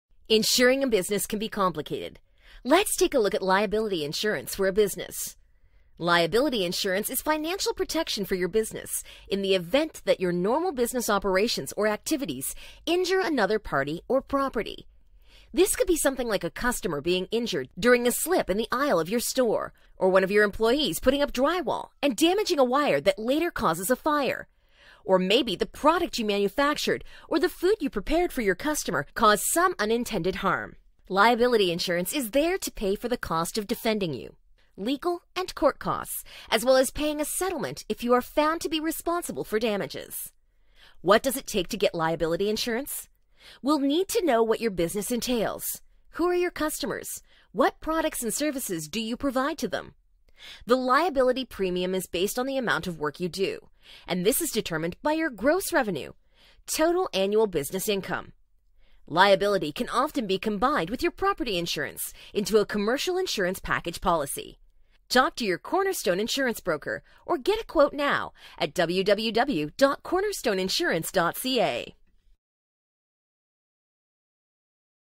Commercial Liability Explained
Liability insurance can be complicated.  Watch as coverage is explained and simplified.  Tips on what you will need to get started.  Contact a Commercial Lines Broker for more information.